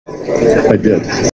Sweeney in isolation
The question:  did you give honest answers to those questions?

I ran this response against Lie Detector Voice - a free Android app.

It concluded this was a lie.

You ought to try it yourself.